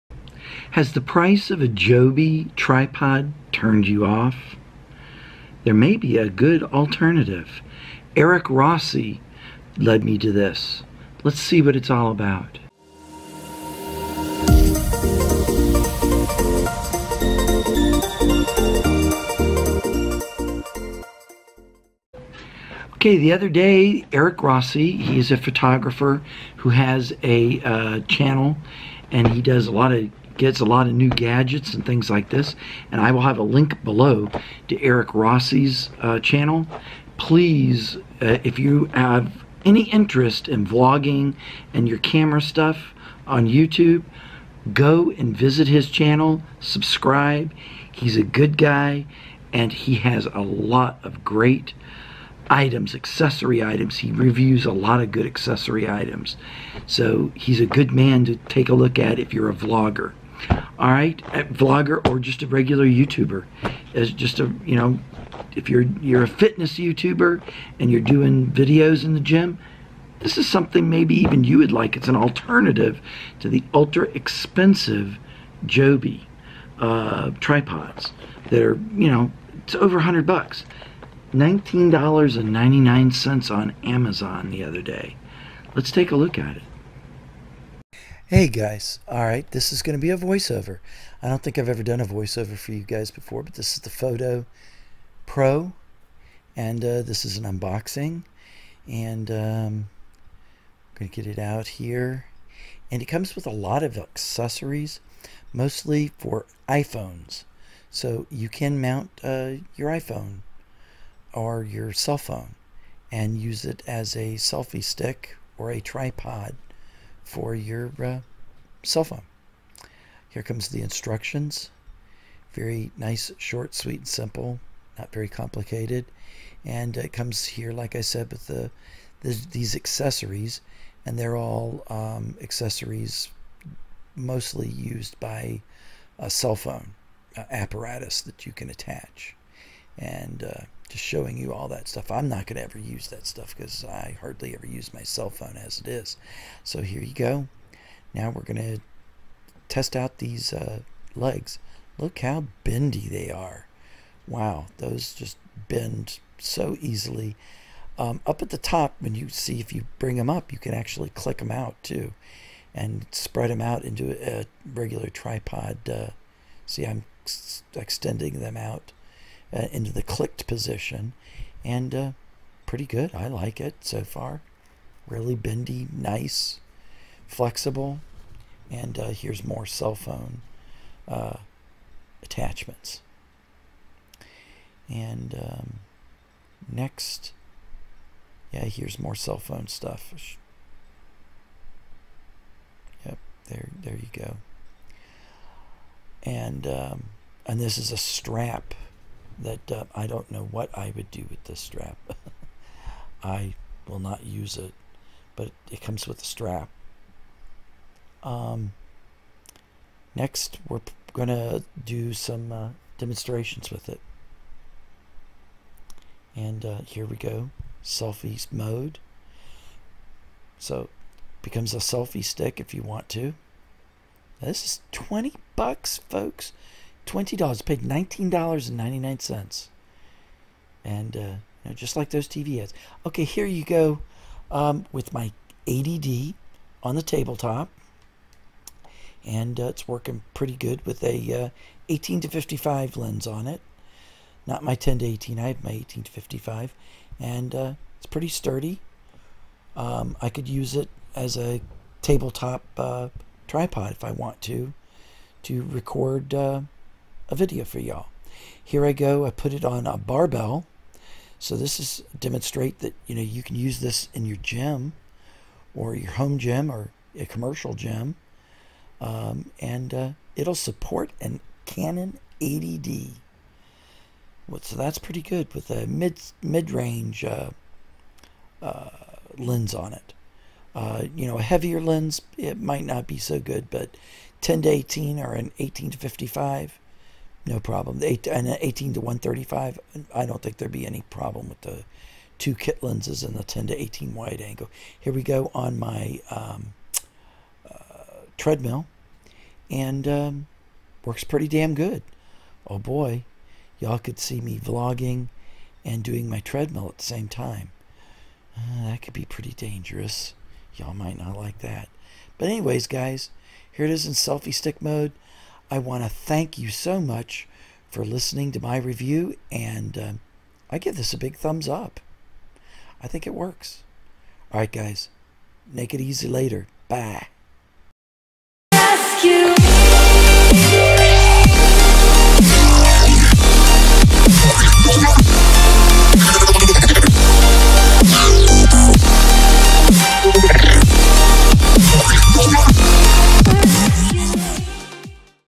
Is this $20 Tripod as good as a $150 JOBY GORILLAPOD ? A review of the Fotopro UF2 Tripod
My review of the Fotopro UF2 Tripod. Sells for $20 on Amazon. Suggested to me by my friend Eric Rossi.

Amazon link to Fotopro Tripod